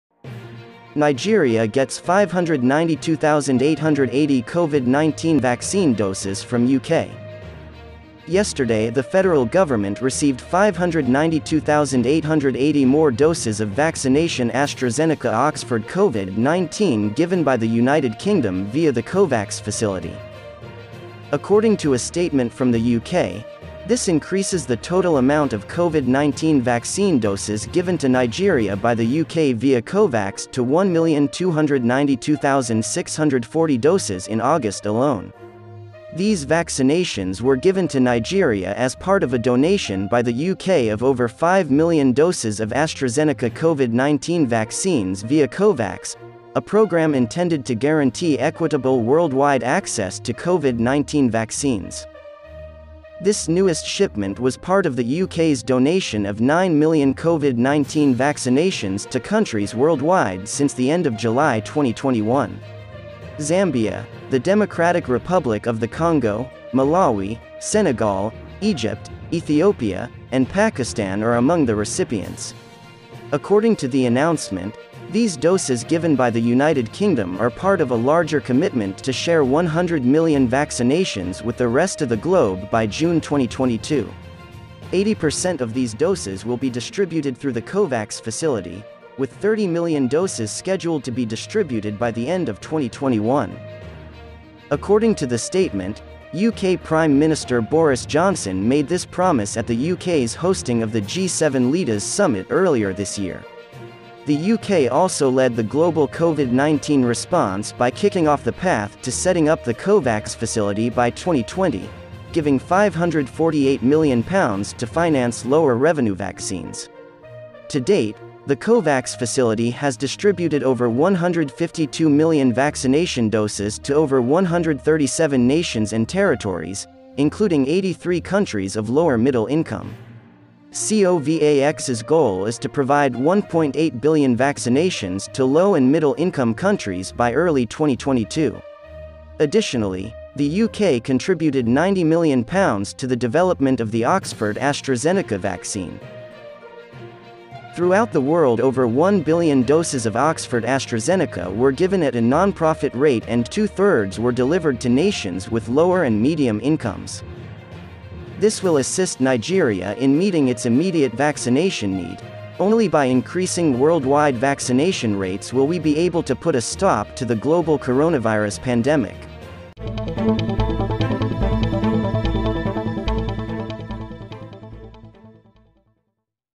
Nigeria Gets 592,880 COVID 19 Vaccine Doses from UK
Yesterday the federal government received 592,880 more doses of vaccination AstraZeneca-Oxford COVID-19 given by the United Kingdom via the COVAX Facility.   According to a statement from the UK, this increases the total amount of COVID-19 vaccine doses given to Nigeria by the UK via COVAX to 1,292,640 doses in August alone.